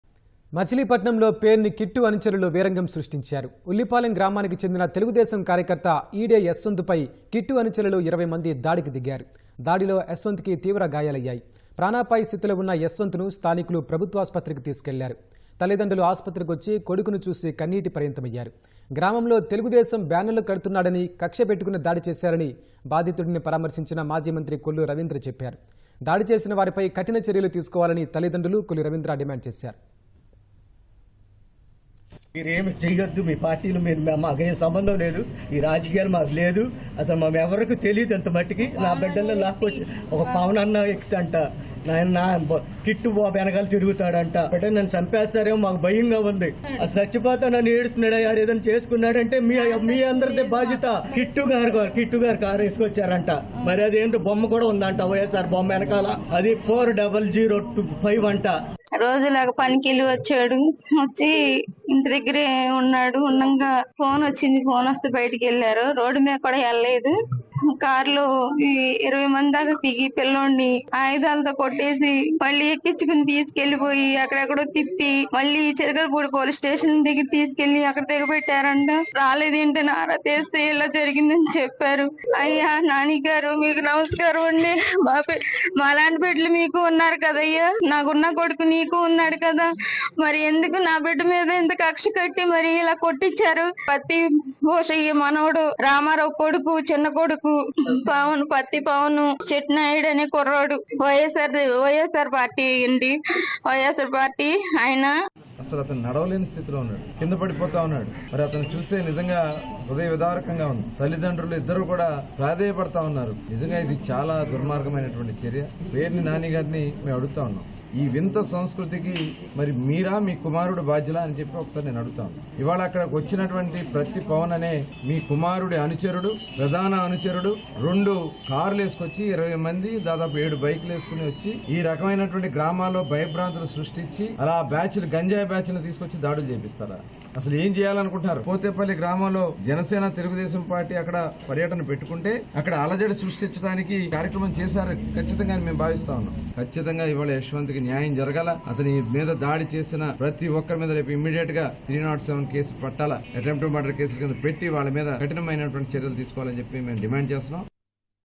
తెలుగుదేశం కార్యకర్త పై దాడి | Attack on TDP Activist | Machilipatnam
మచిలీపట్నంలో పేర్ని కిట్టు అనుచరులు వీరంగం సృష్టించారు. ఉల్లిపాలెం గ్రామానికి చెందిన తెలుగుదేశం కార్యకర్త ఈడే యశ్వంత్ పై...కిట్టు అనుచరులు 20 మంది దాడికి దిగారు. దాడిలో యశ్వంత్ కి తీవ్ర గాయాలయ్యాయి. ప్రాణాపాయ స్థితిలో ఉన్న యశ్వంత్ ను స్థానికులు ప్రభుత్వాసుపత్రికి తీసుకెళ్లారు. తల్లిదండ్రులు ఆస్పత్రికి వచ్చి కొడుకుని చూసి కన్నీటిపర్యంతం అయ్యారు. గ్రామంలో తెలుగుదేశం బ్యానర్లు కడుతున్నాడని కక్ష పెట్టుకుని దాడిచేశారని...బాధితుడిని పరామర్శించిన మాజీ మంత్రి కొల్లు రవీంద్ర చెప్పారు.దాడి చేసిన వారిపై కఠిన చర్యలు తీసుకోవాలని తల్లిదండ్రులు, కొల్లు రవీంద్ర డిమాండ్ చేశారు..